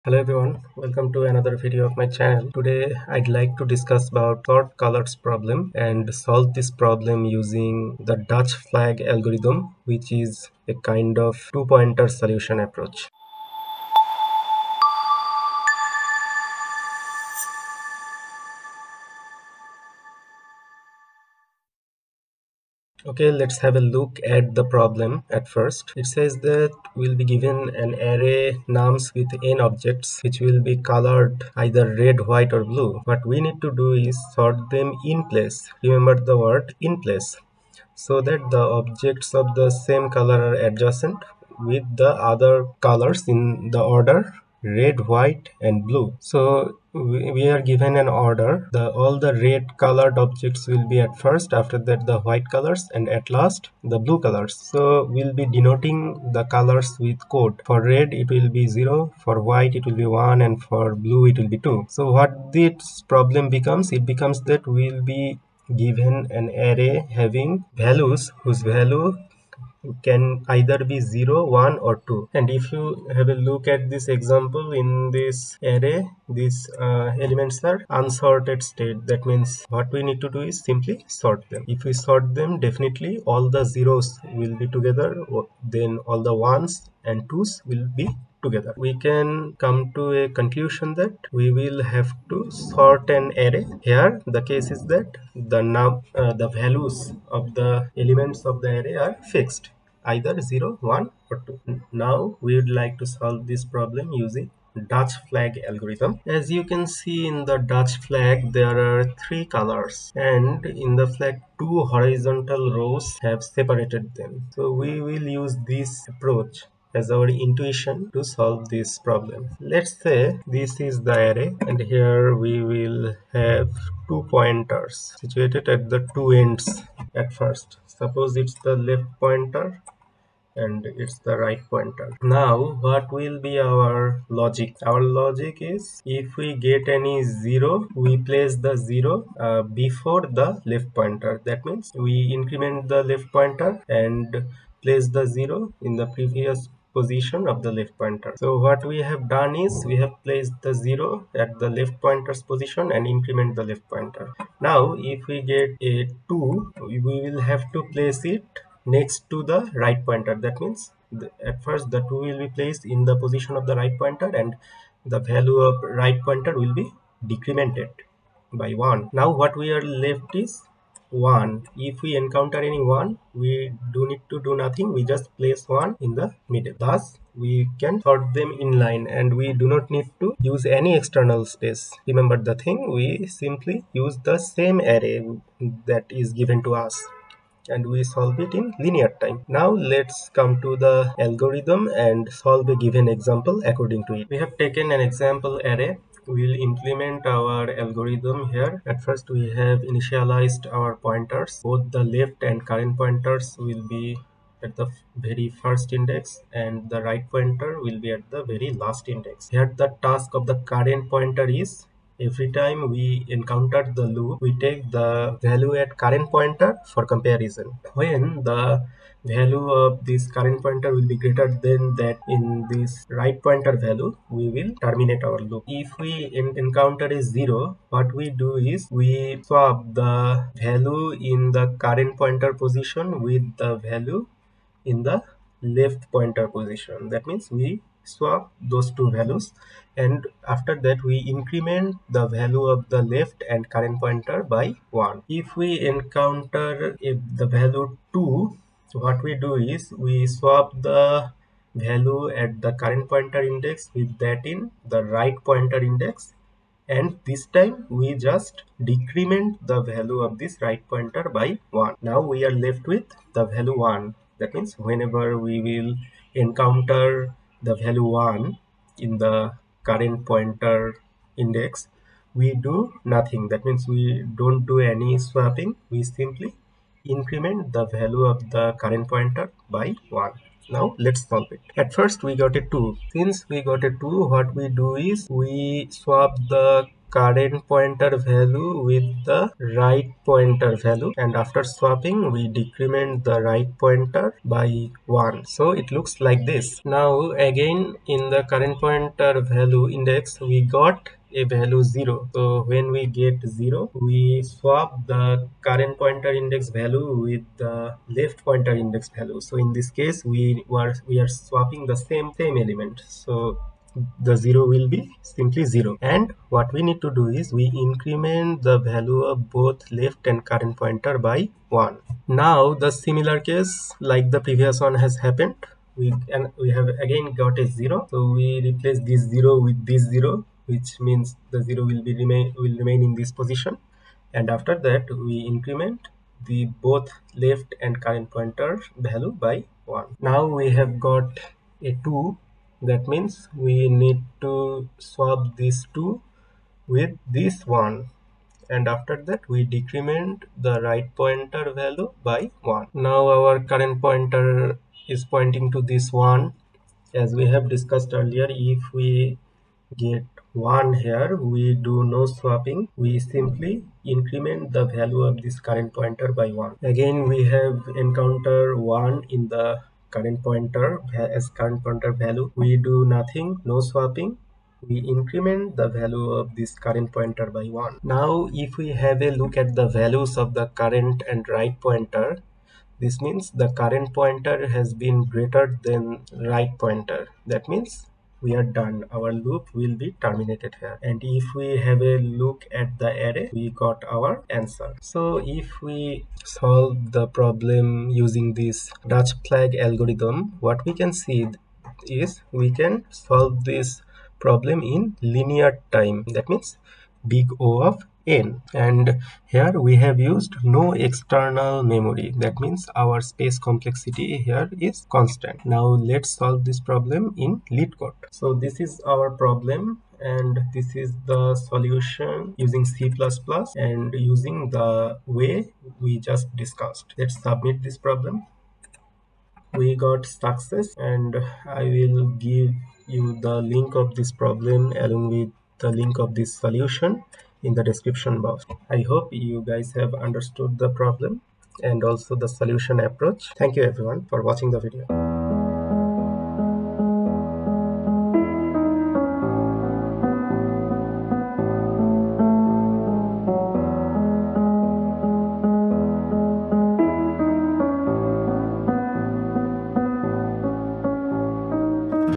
Solving Colors Sort Problem Using Dutch Flag Algorithm
In this video I have tried to solve the colors sort problem using dutch flag algorithm which is basically a two pointer approach. This video contains the following sections:
0:00 Introduction
0:23 Problem Statement
1:49 Solution Approach
3:14 Simulation
6:58 C++ Implementation
7:25 Conclusion